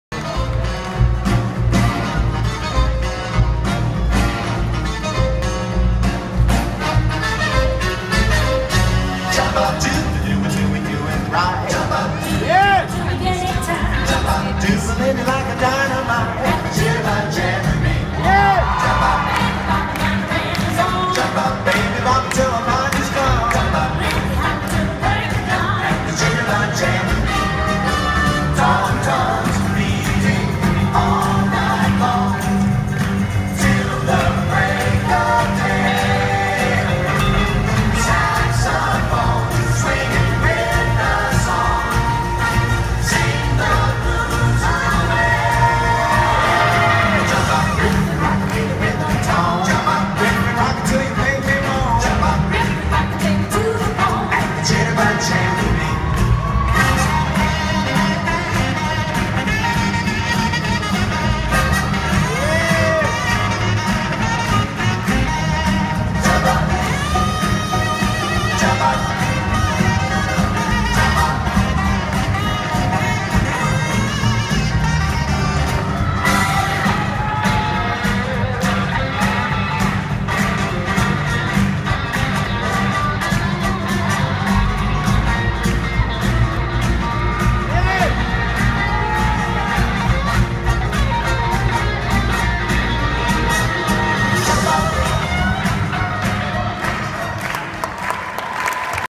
World Championship Boogie Woogie 2015 Heat 18 Vegard & Ingunn
First Round - Heat 18
Vegard Stengrudet & Ingunn Molde and Nikolay Khitruk & Evgenia Khavtorina